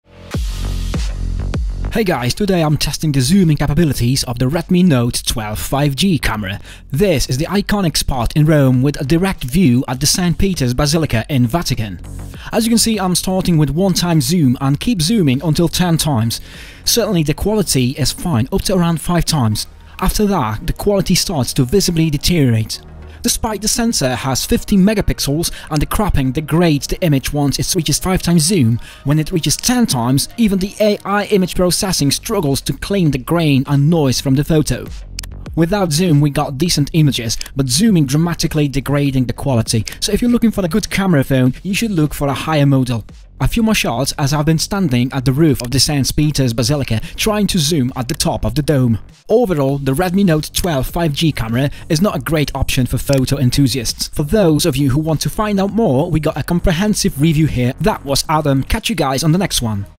REDMI NOTE 12 5G Zoom Test: See the World Up Close (But Don't Expect Great Photos)
The Redmi Note 12 5G has 50MP main camera, but can it zoom in to 10x and still take good photos? Find out in this video!

For the full, comprehensive review which covers everything you'd want to know about this mid-range low-tier phone, please go to:

Thanks to our awesome Fans and Family for support!